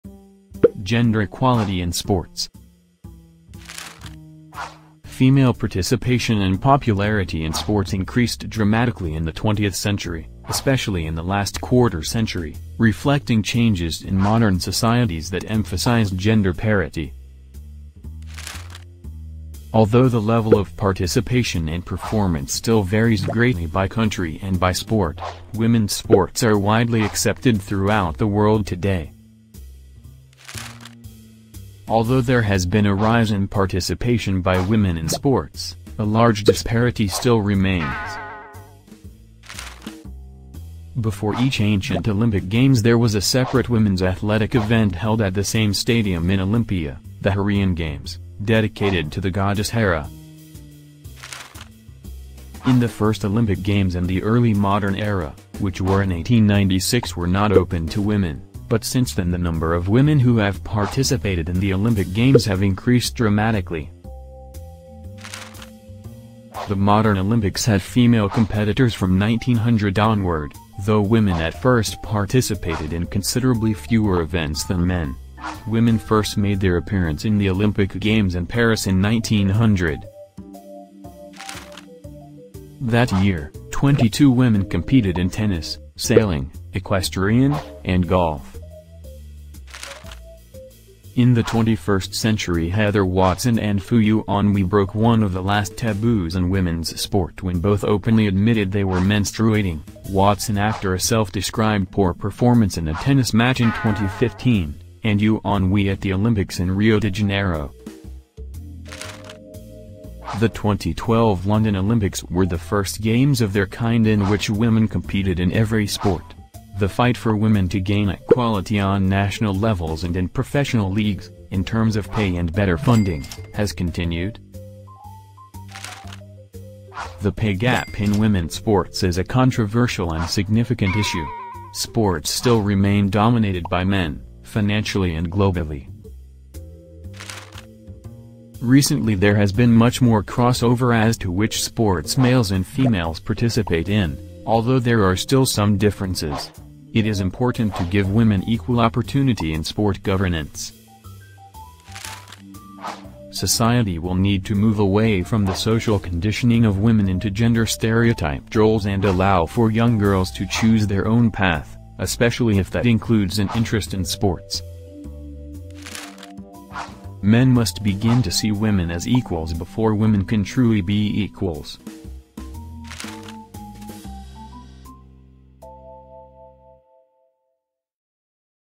Gender Equality and Sports
Female participation and popularity in sports increased dramatically in the twentieth century, especially in the last quarter-century, reflecting changes in modern societies that emphasized gender parity. Although there has been a rise in participation by women in sports, a large disparity still remains.

This video was produced by: Alexandra Sutoiu. Responsibility for the accuracy of the content provided resides solely with the authors.

This video was created in the context of the UNSSC and simpleshow foundation Volunteer Initiative